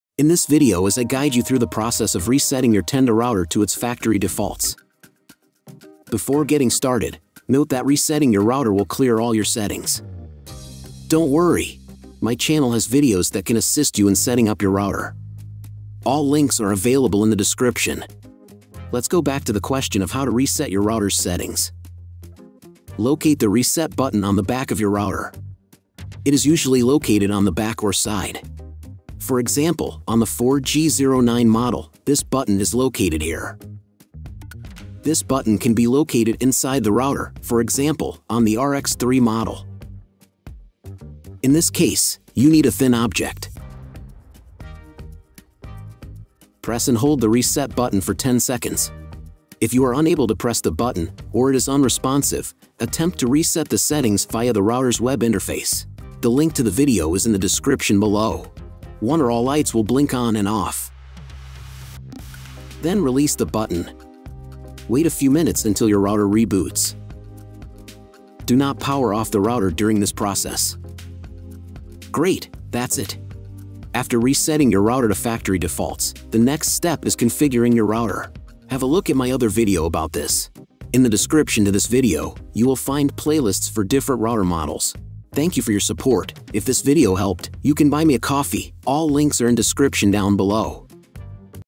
🔁 How to Reset a Router | TENDA
In this video, I'm gonna show you, how to reset your Tenda router to the factory default settings.

Why do you need to reset a router?
- If you cannot log in to the router's web interface.
- If you forgot Tenda router password.
- If you have problems with your Internet connection.
- If you want to sell your router.
In all of these cases, you need to reset router to the factory settings.

🟡BTC: bc1q6lxzusxt27nl25fwcluw7g72uveat2gr7q4as7
🟡ETH: 0xA93CabcE97Ec2c20fc08382283a2CC95d6579EFB
🟡USDT (TRC20): TANkFPkipQ66cFacAe85PiZSq6cEJfBBRQ
🟡USDT (ERC20): 0xA93CabcE97Ec2c20fc08382283a2CC95d6579EFB